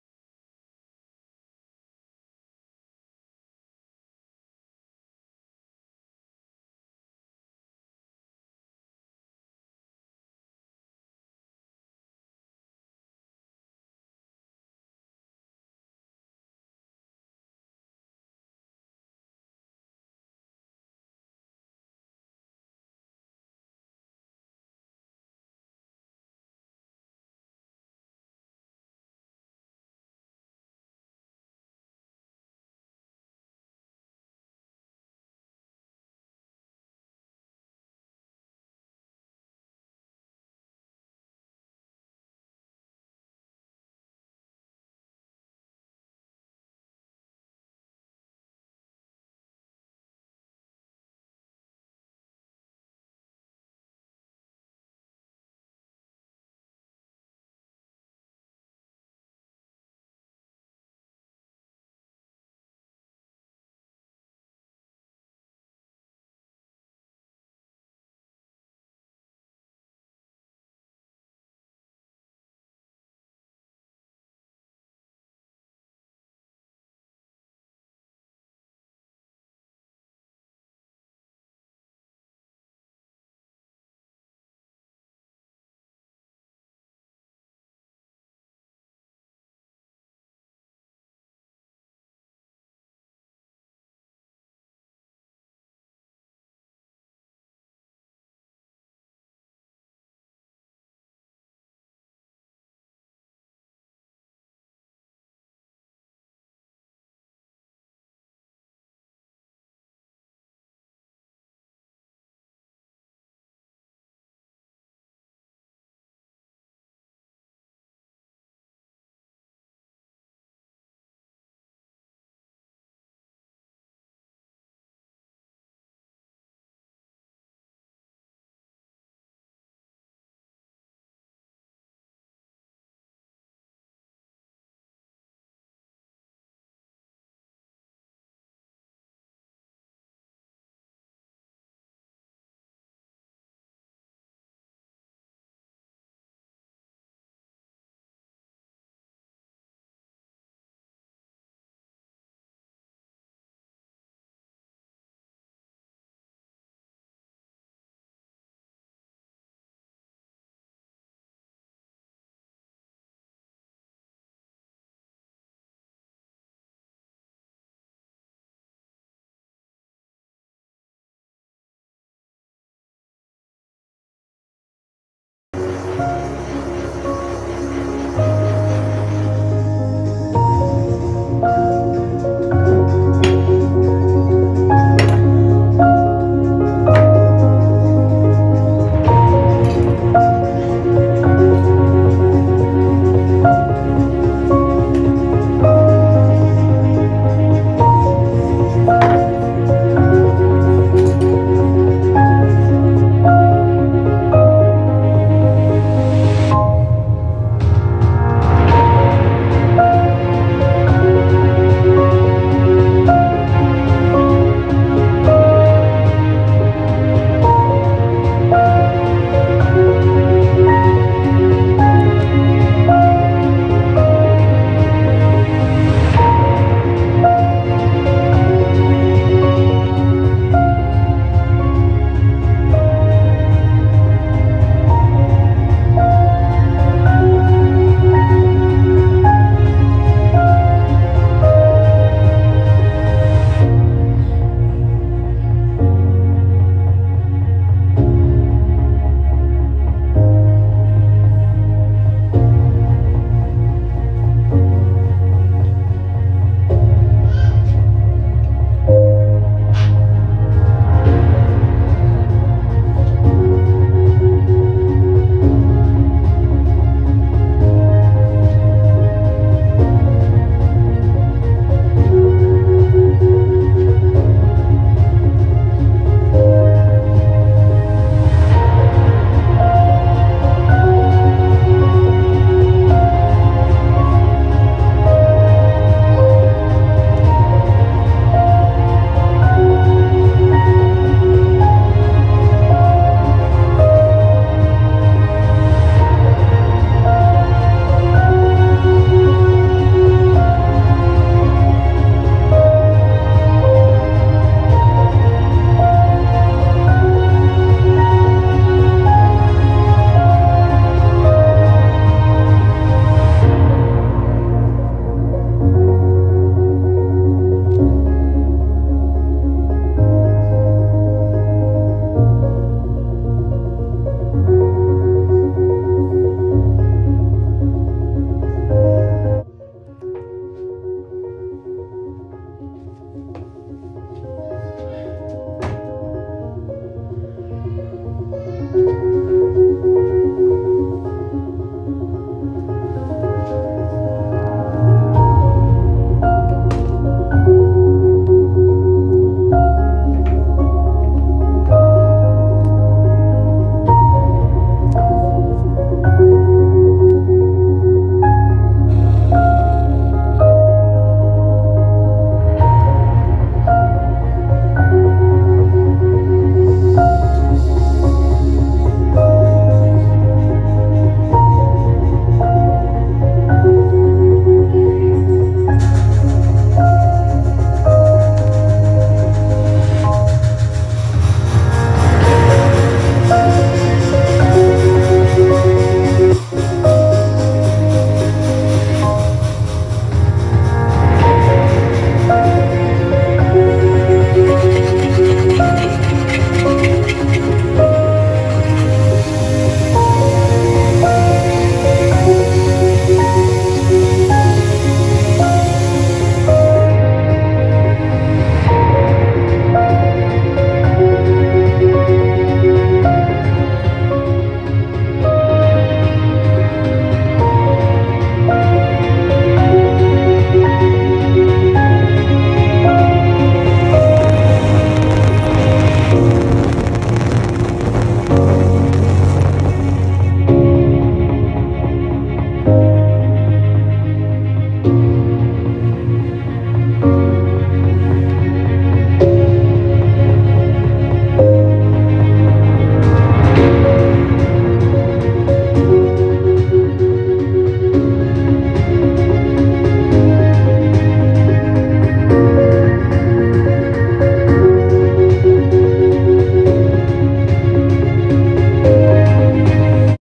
incredibly beautiful ring made using filigree technique with pearls
Hello friends, in this video I show the filigree manufacturing technology.  Happy viewing, I hope this video will be useful.